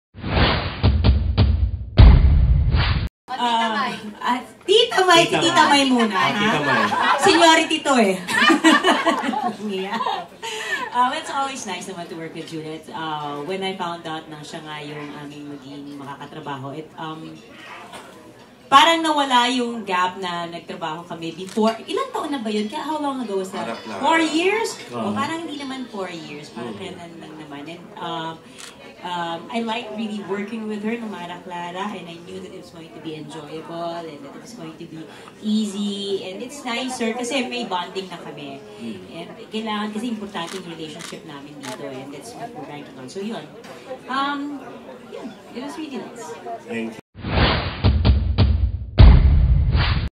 Mylene Dizon can't believe it's been 4 years since she last worked with Julia Montes
Mylene Dizon says bonding between her and Julia Montes is important.